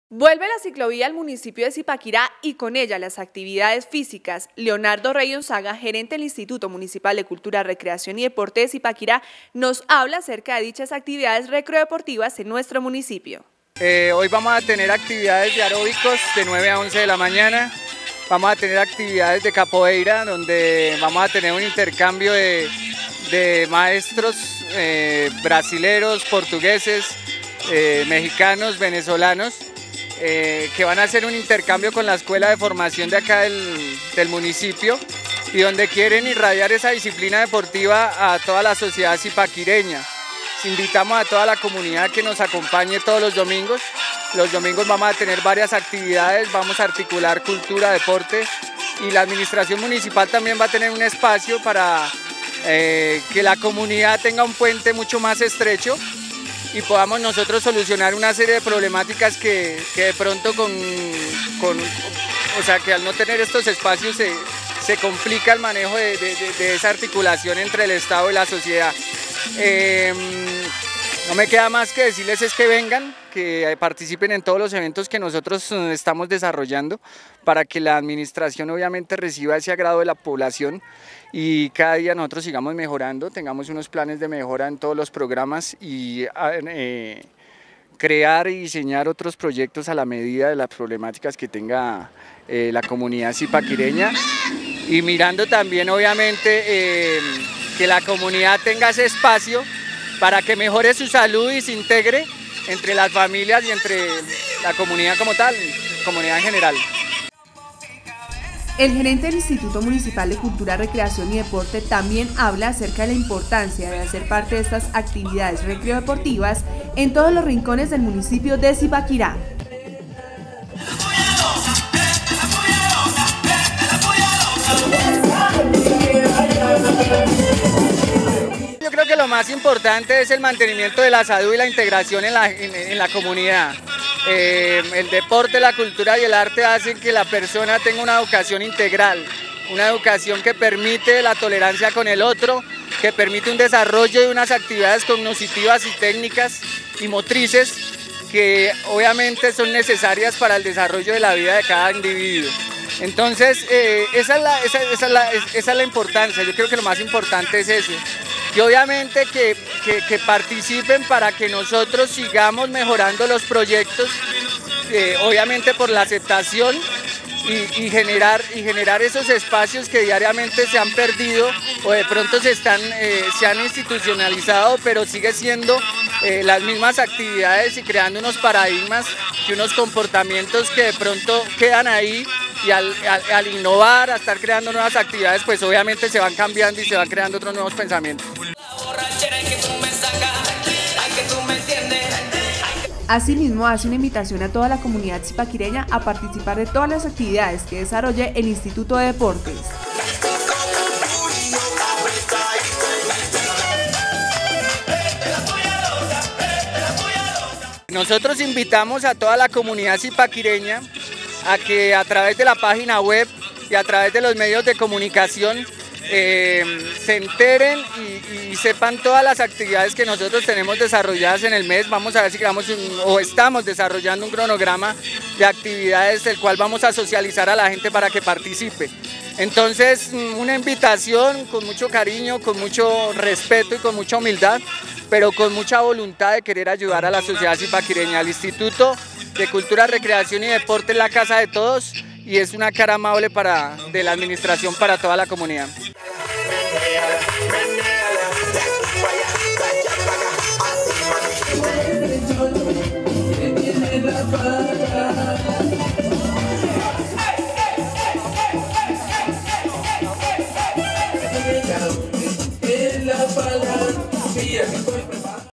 RE-APERTURA DE LA CICLOVÍA Y ACTIVIDADES RECREODEPORTIVAS EN ZIPAQUIRÁ
Con actividades como aeróbicos y capoeira se llevo a cabo el pasado domingo 10 de enero la re apertura de la ciclovía, en la cual se piensa vincular a la comunidad con la cultura y el deporte, con el fin de dar solución a las diferentes problemáticas, integrando a las familias y mejorando la salud.